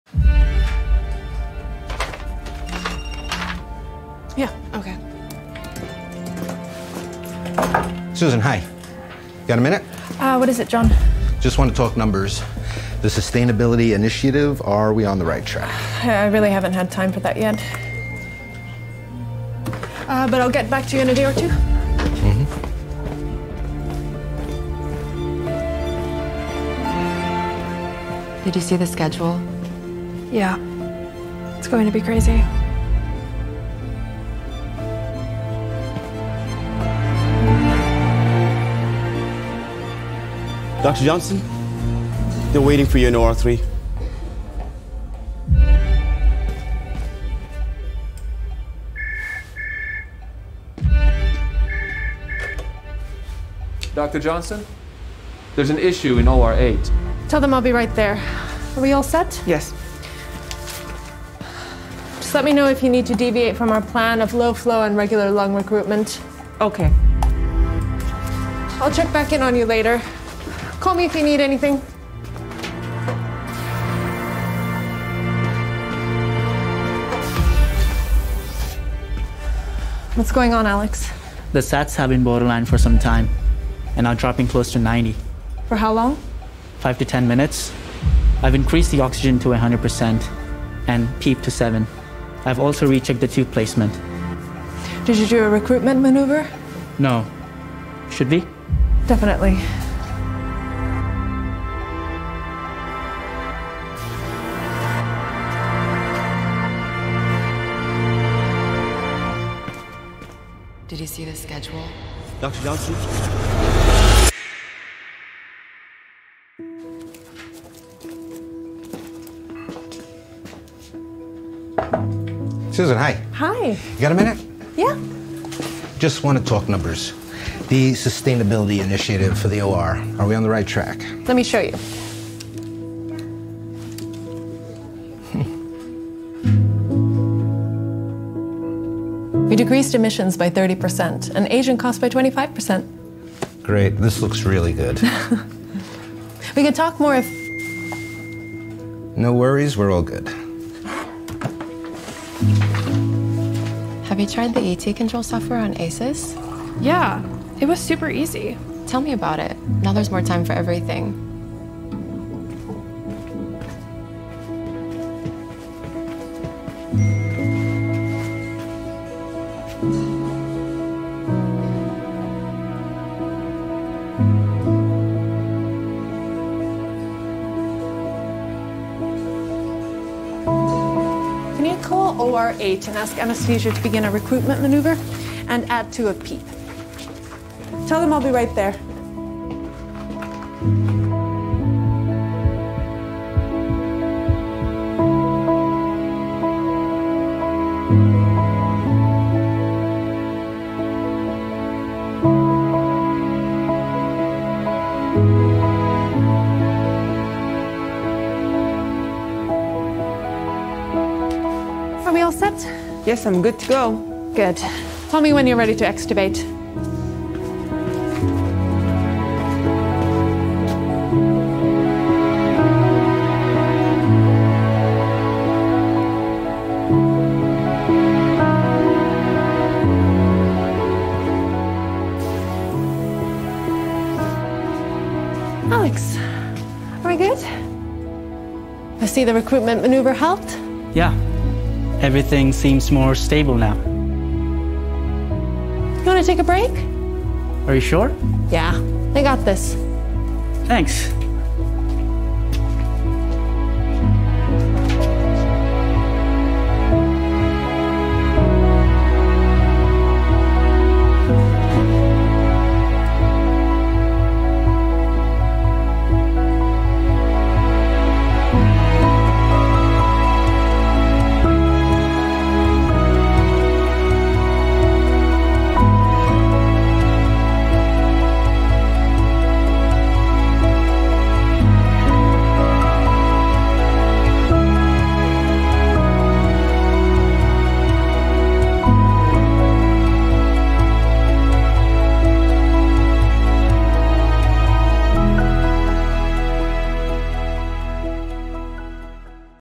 Aisys CS² Anaesthesia Delivery System with Et Control: easing workflows - nonUS
Watch the story of how Dr. Johnson, Chief of Anaesthesiology, transformed her hectic day into a streamlined workflow that helped ease the way to more confident patient care using the Aisys™ CS² Anaesthesia Delivery System with End-tidal Control. See how she improved operating room efficiencies when she combined the Aisys anaesthesia machine with Carestation™ Insights Applications. Working in this intelligent OR ecosystem she was able to practice low-flow anaesthesia with confidence to help support her sustainability goals for a greener OR. Dr. Johnson is a fictional character.

GE is a trademark of General Electric Company used under trademark license. Reproduction in any form is forbidden without prior written permission from GE HealthCare.
JB23212XX